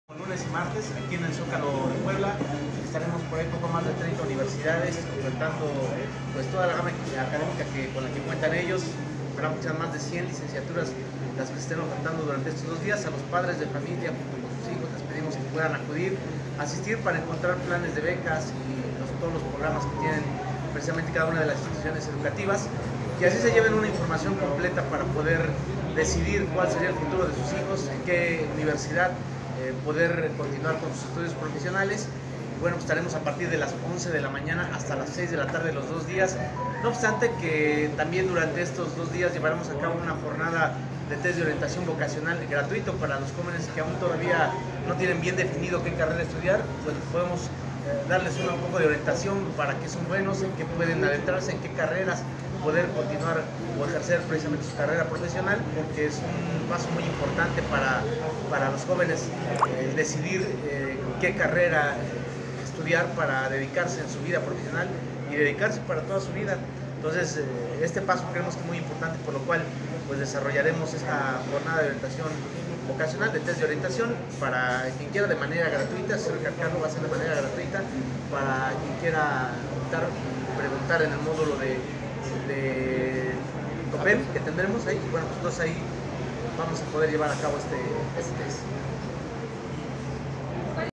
HABRÁ UNA JORNADA DE ORIENTACION VOCACIONAL GRATUITA
Con la finalidad de ofrecer a los jóvenes que no alcanzaron lugar dentro de alguna institución de educación superior pública, Grupo Comunicación Publicitaria Empresarial (COPEM) realizará en el zócalo de Puebla el Encuentro Profesional con Universidades (EPCU), este lunes 15 y martes 16 de julio.

En rueda de prensa, los organizadores del EPCU 2024, destacaron que con esta actividad se busca apoyar a los jóvenes que egresan del nivel medio superior y desean continuar con su preparación profesional y que no alcanzaron lugar en alguna de las licenciaturas que ofrece la máxima casa de estudios. Valentín Paredes Hernández, destacó que cerca de 30 universidades ofrecerán becas, descuentos a los padres de familia y jóvenes para que continúen con sus estudios universitarios.

"Con este tipo de acciones, queremos que quienes no alcanzaron lugar en nuestra máxima casa de estudios, no se desanimen y busquen otra opción universitaria, con becas y descuentos", destacó Paredes Hernández. En su momento, Ricardo Carmona de Jesús, agregó que, Grupo COPEM ofrece a los jóvenes de formna gratuita un test de orientación vocacional y de esta forma reafirmen la licenciatura o área de interés y con ello evitar los indices de deserción, que diferentes casas encuestadoras se calcula que cerca del 40 por ciento de quienes desertan en su mayoría lo hacen por la falta de orientación vocacional.

Invito a los padres de familia y a los jóvenes que conozcan las opciones que las universidades les ofrecerán el lunes 15 y martes 16 de julio, "Buscamos que en familia los jóvenes puedan elegir una licenciatura y no trunquen su preparación universitaria".

Finalmente, destacó que entre las universidades participantes se encuentran: La Universidad Interamericana, COMCIPE, Universidad de los Ángeles, Rudolf Diesel, Instituto Universitario Puebla (IUP), Universidad del Valle de Puebla, El Instituto de Estudios Universitarios (|EU), entre otras instituciones de educación superior.